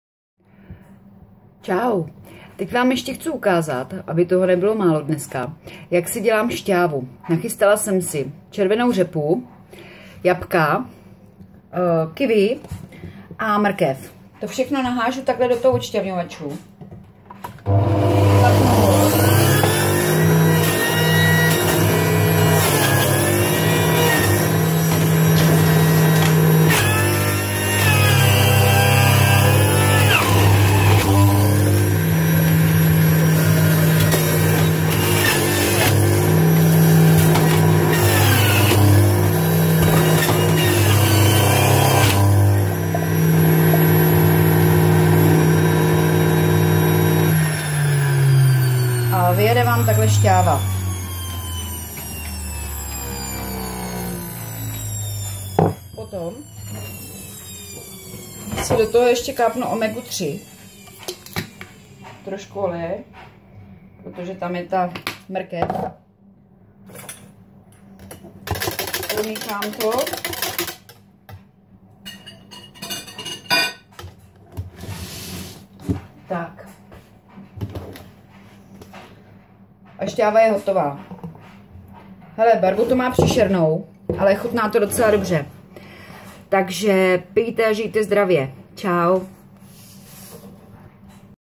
Rakovina prsu / Domácí džusy ...
Jak vyrobit domácí džus...

Má videa nejsou návodem na přežití,ale ukázka toho jak se dá žít!